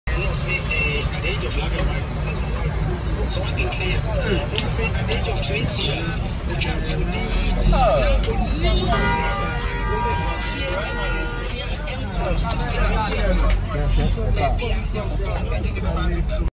Dramatic Accident on the Tema highway involving 8 cars
An accident which occurred on the Tema Highway today 03/12/2020, involving 8 cars. Number of death or injuries are not recorded yet.